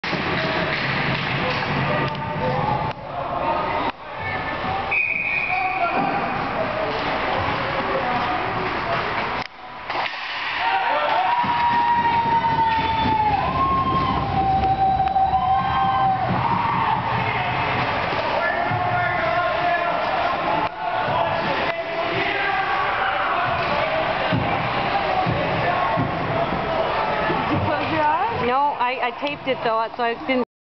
Michael Comitini vs Tilton
Michael Comitini stops penalty shot